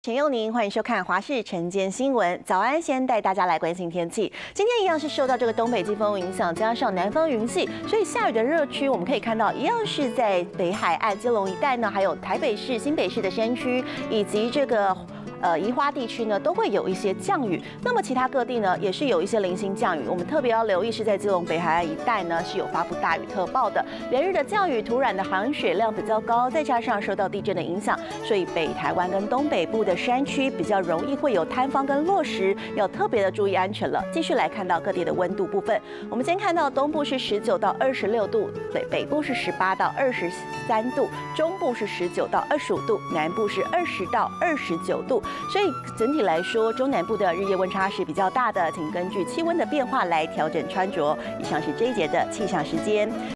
基隆.新北大雨特報! 留意土石鬆動｜華視新聞 20201211
今天為東北季風環境加上仍有南方雲系影響，下雨的熱區還是在大臺北地區、基隆北海岸至東半部一帶為主，其他各地也會有有零星降雨；其中特別留意在基隆北海岸仍有局部大雨發生的機率，連日降雨，土壤含水量已高，再加上地震的影響，北臺灣及東北部山區易有坍方及落石，要特別注意安全；繼續來看到各地溫度，東部19-26，北部 18-24，中部19-25，南部20-29，南部地區日夜溫差較大，請根據氣溫變化適時調整穿著。